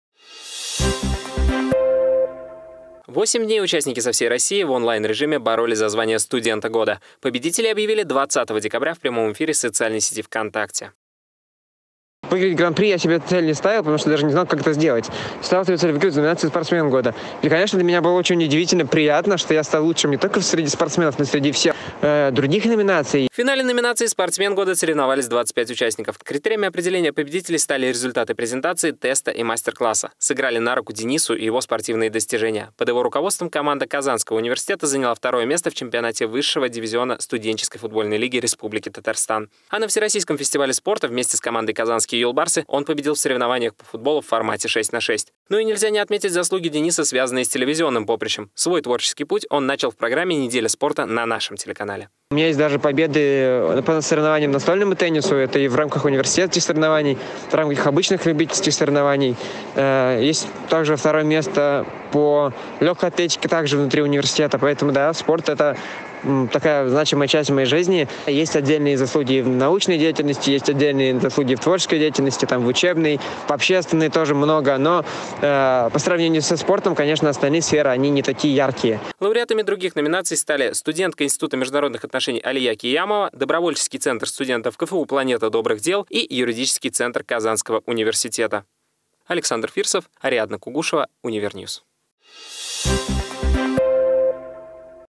МАГИСТРАНТ КФУ СТАЛ СТУДЕНТОМ ГОДА
В Москве подвели итоги российской национальной премии "Студент года - 2021". Кто из учащихся Казанского федерального университета получил гран-при, расскажем далее.